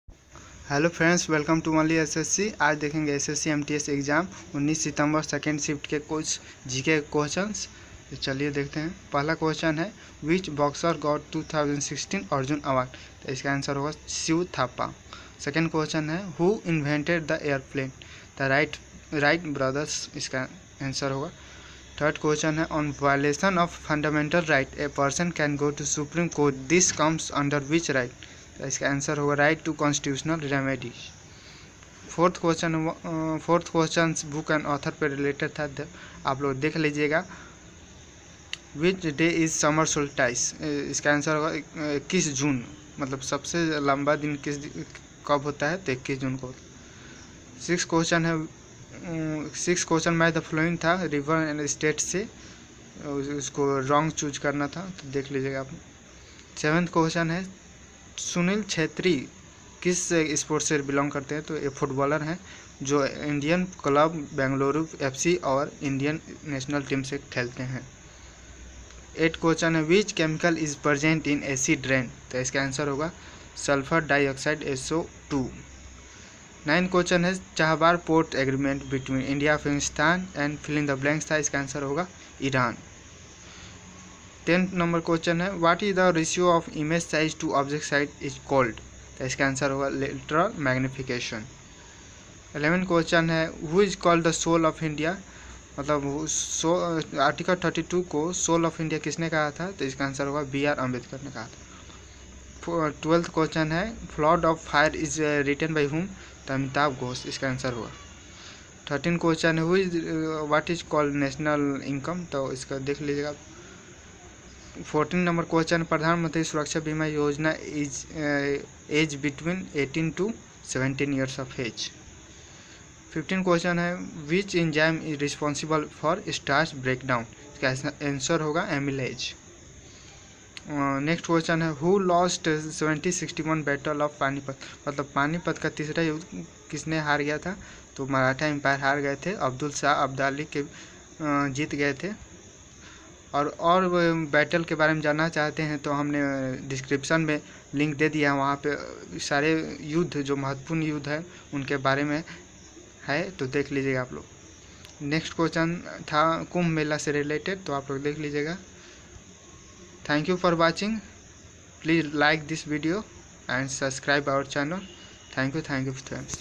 SSC MTS EXAM 19 September 2017 2nd SHIFT Asked GK QUESTIONS
SSC MTS EXAM 19 September 2017 2nd Shift Gk QUESTIONS With solution